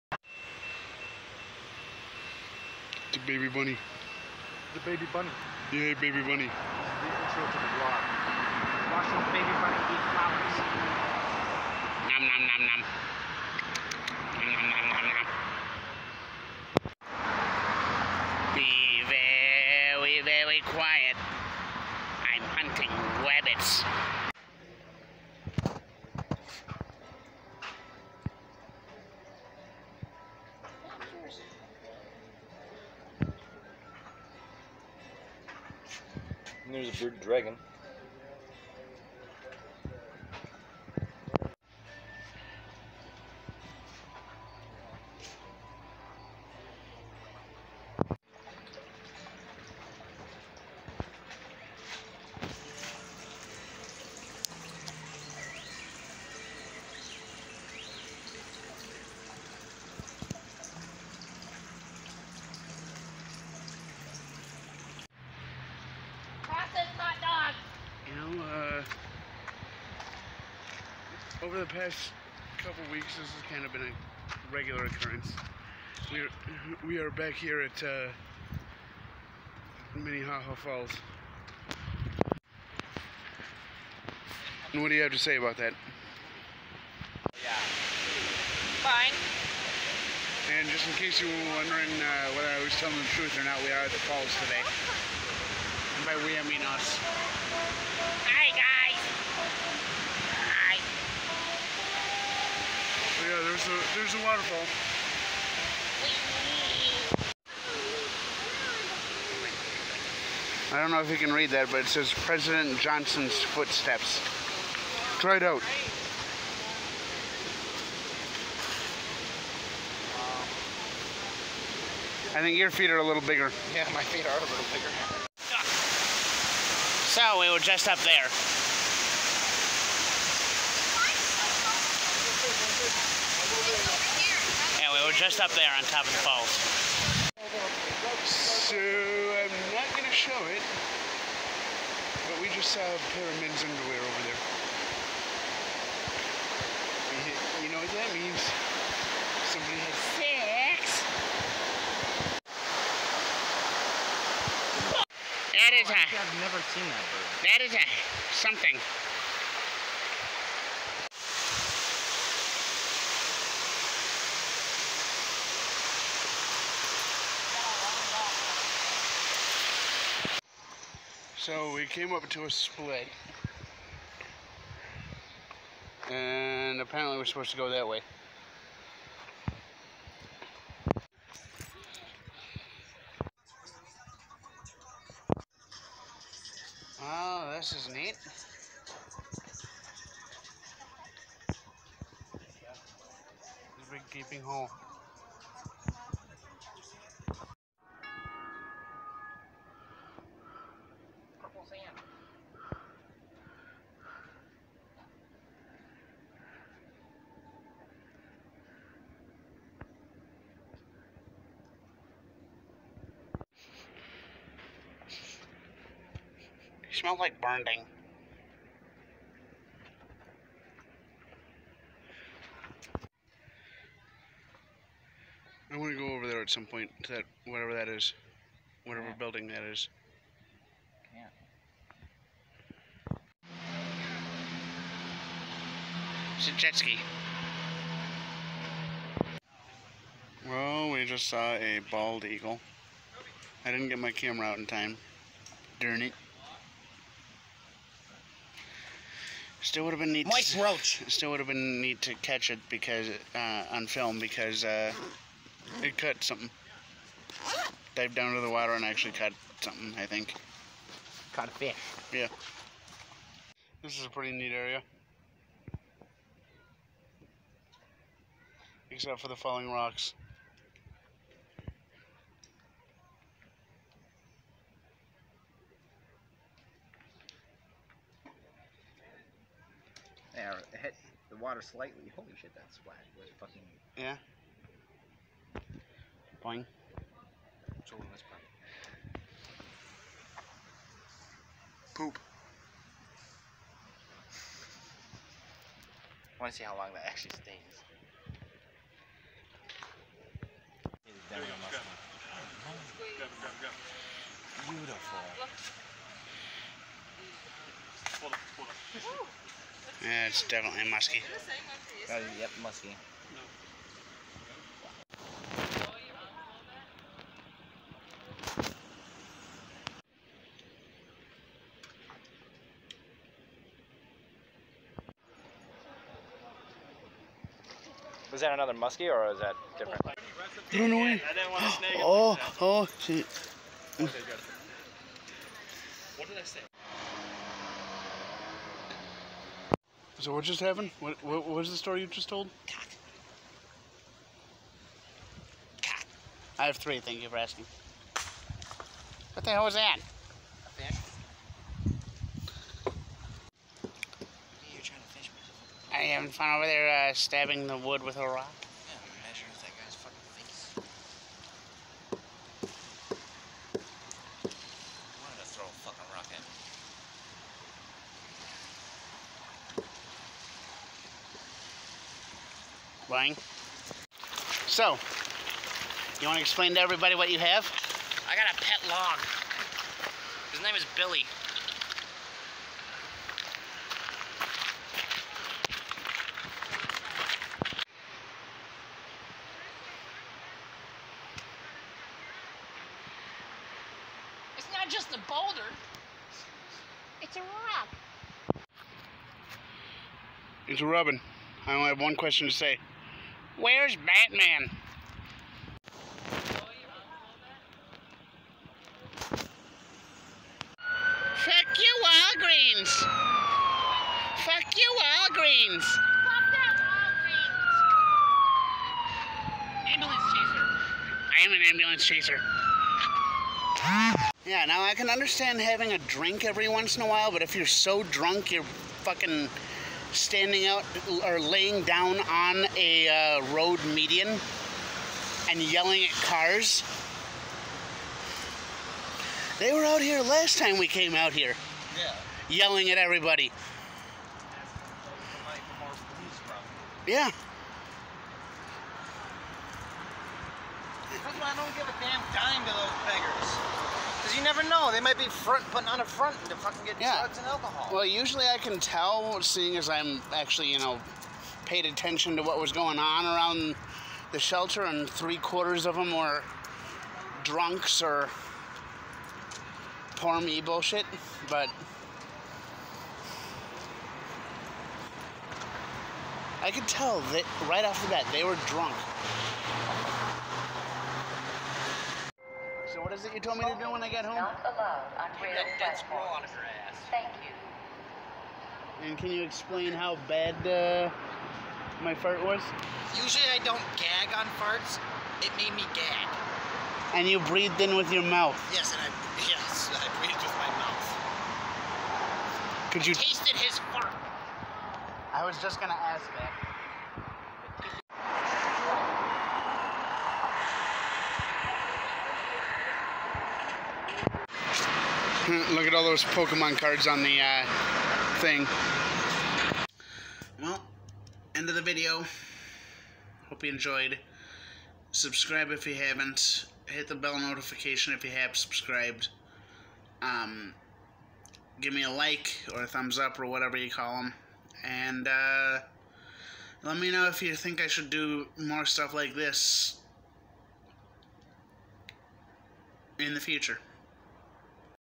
Exploring More of Minnehaha Falls (28 June 2019) Vlog MN
This is the first time I've been to this portion of Minnehaha Falls. What do you think about the footage?